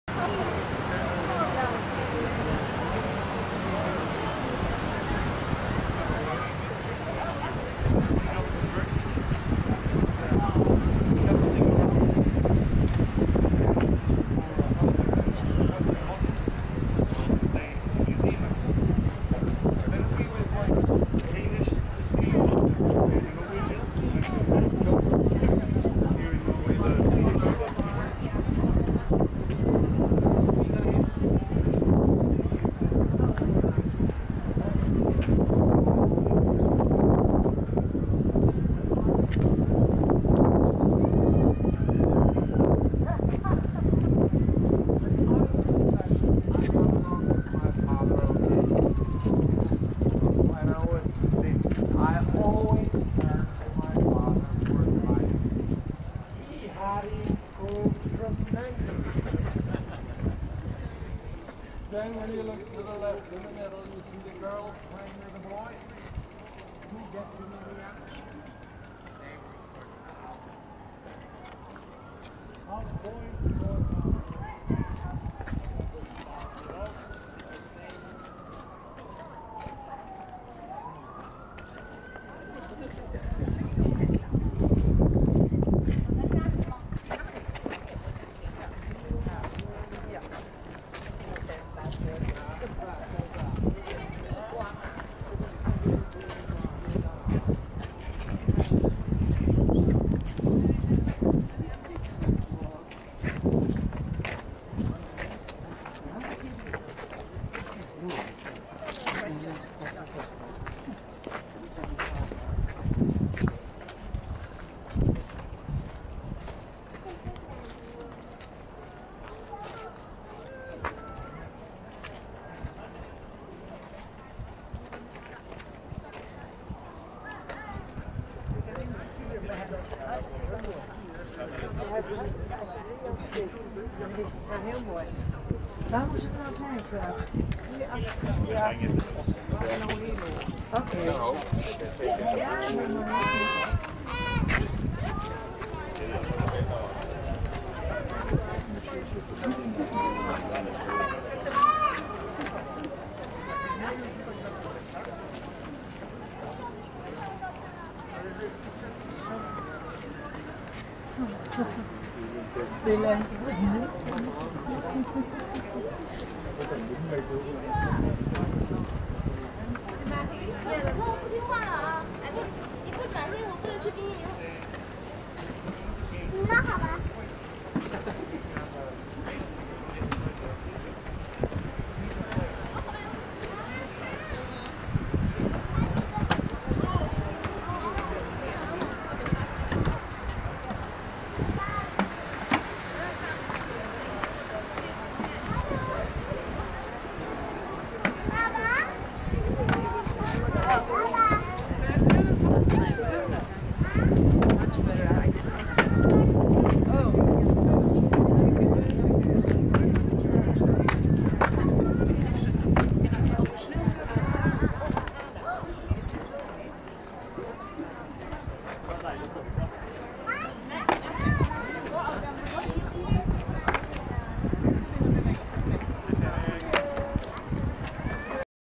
2023.07.20 비겔란 공원 Vigelands Park
북유럽 여행 중 노르웨이 오슬로의 비겔란 공원에서 촬영한 영상입니다.